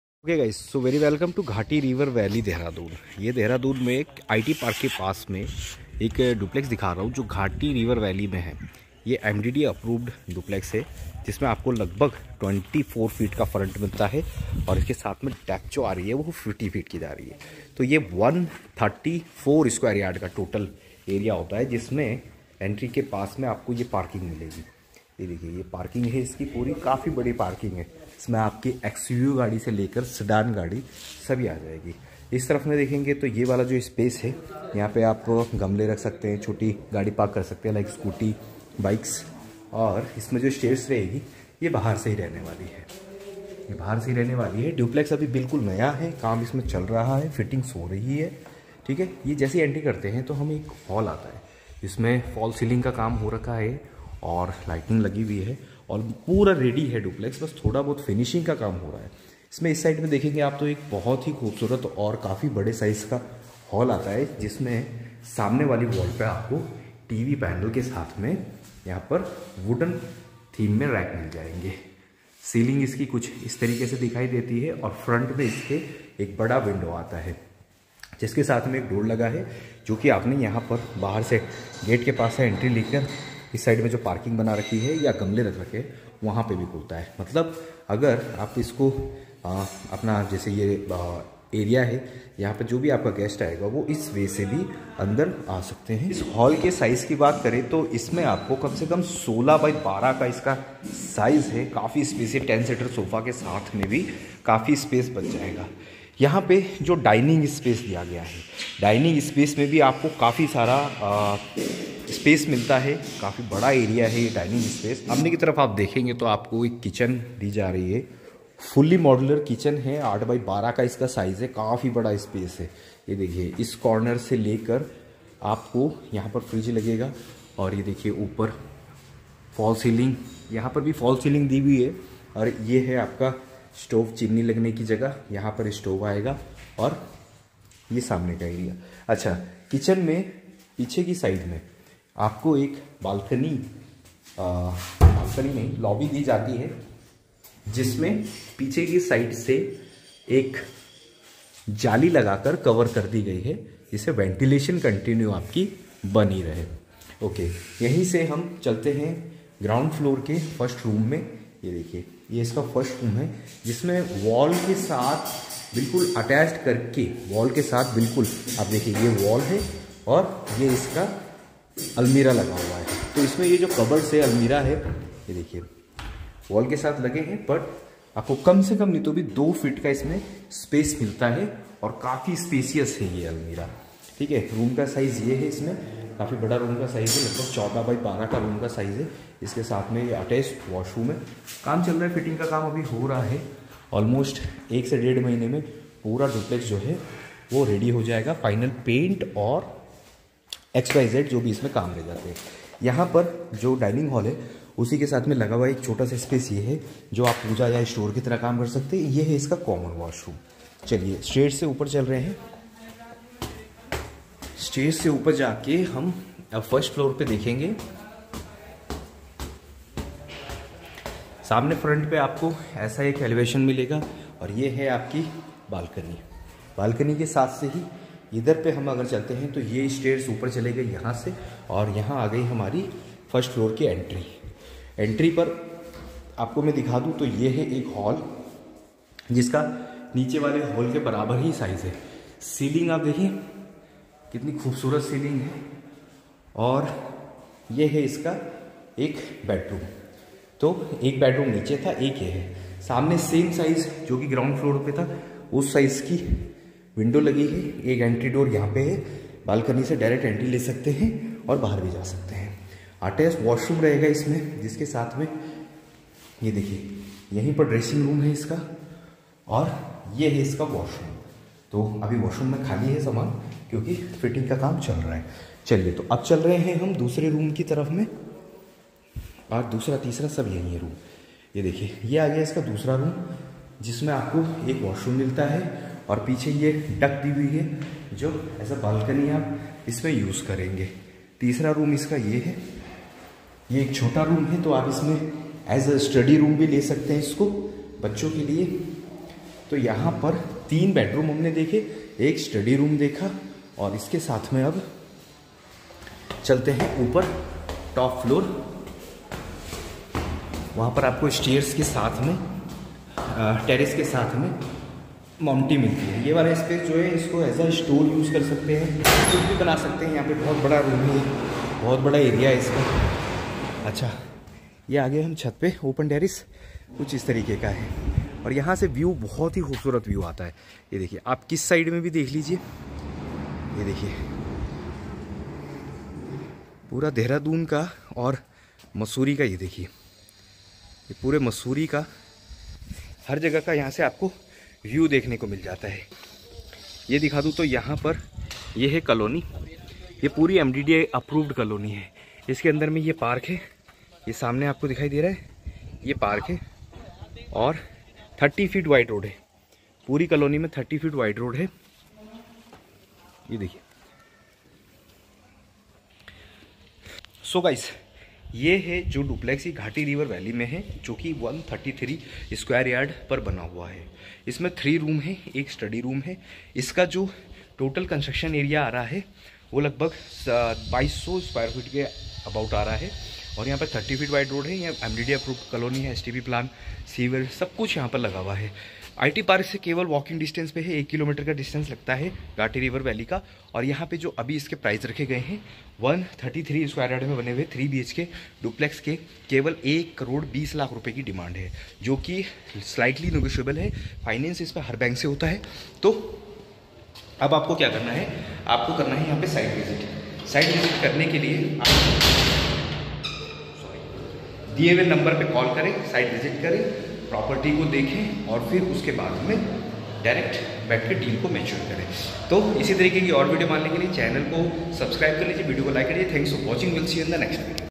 3 BHK + Study Room House for Sale near Sahastradhara Road, Dehradun | Location , Ghati River Valley
Explore this incredible opportunity to own a beautiful 3 BHK house with a study room in the scenic surroundings of Dehradun, near Sahastradhara Road. Located in the prestigious MDDA approved colony, this house is nestled in the serene Ghati River Valley, just a stone's throw away from IT Park.

Key Features:

Address: Ghati River Valley, Sahastradhara Road, near IT Park, Dehradun.
MDDA approved colony
Park available for residents
30 feet wide road for easy access
House dimensions: 24 feet front, 50 feet depth
Spacious 3 BHK + study room
Tranquil surroundings by the Ghati River Valley
This meticulously designed home offers a perfect blend of modern living in a peaceful environment. With a spacious 3 BHK layout, including a study room, you'll have all the space you need for your family. The proximity to IT Park ensures convenience and access to all necessary amenities.

The presence of a park within the colony adds to the charm of this property, providing a space for relaxation and leisure. The 30-feet wide road ensures hassle-free movement in and out of the neighborhood.

Don't miss this incredible opportunity to make this house your dream home. For more information or to schedule a viewing, please contact [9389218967]. Experience a quality lifestyle in the heart of Dehradun - a tranquil paradise by the Ghati River Valley